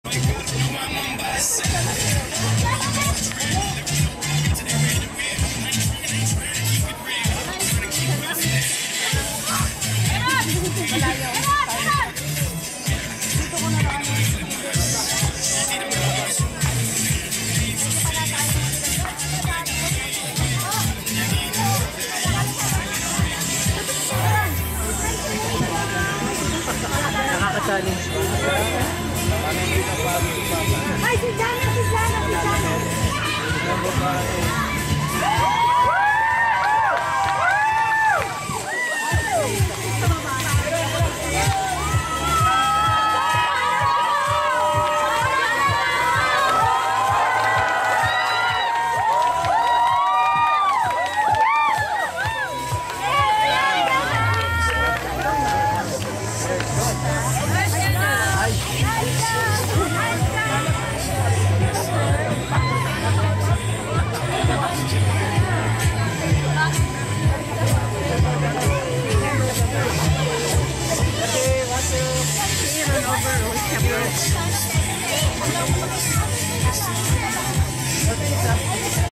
5th Anvaya Cove Invitational Triathlon - Day 1 SuperTriKids finish line
At the finish line during the SuperTriKids (STK) races held on October 20, 2012 in the afternoon at Anvaya Cove Beach & Nature Club in Morong, Bataan.